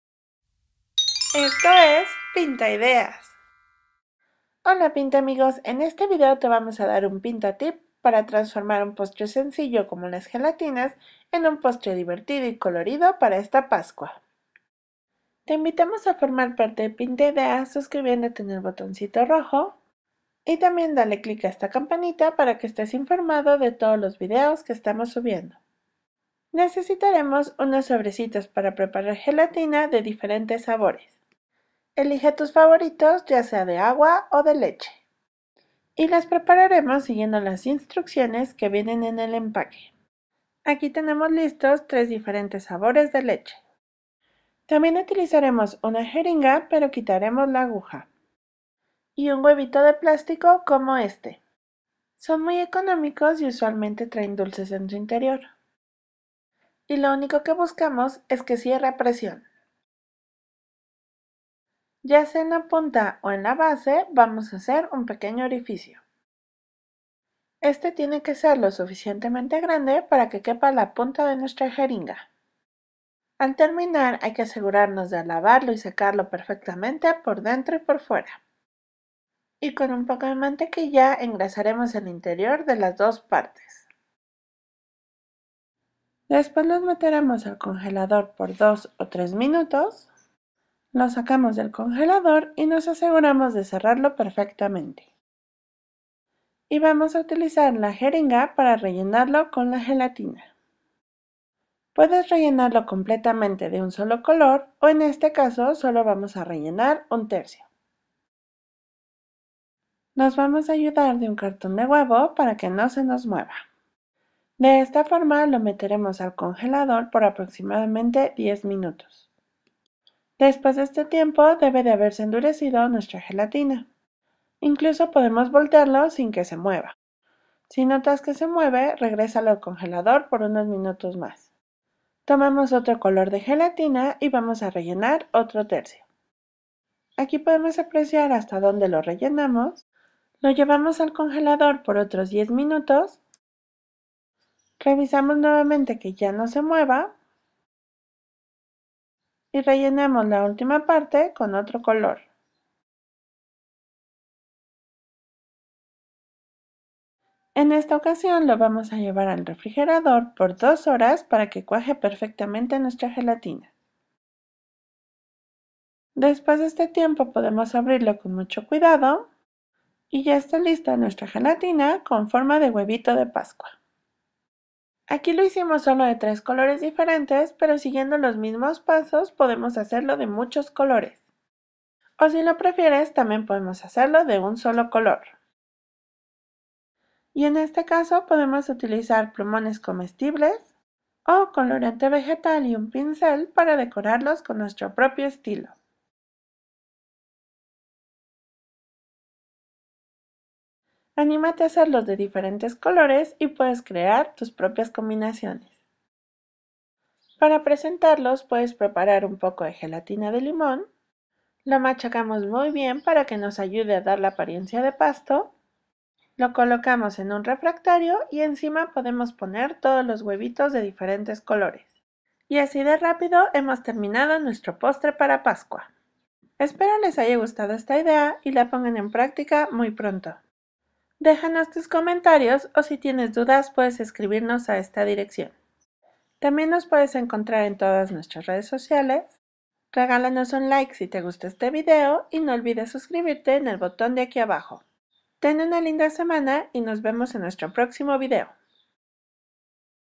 Huevos de pascua de gelatina, Jelly Easter Eggs
En este vídeo te enseñamos a hacer huevos de pascua de gelatina, son super fáciles, con ellos sorprenderás a tus invitados en estas fiestas, puedes hacerlos con tus hijos y pasaras muchos ratos de diversión agregando los colores y sabores que mas les gusten.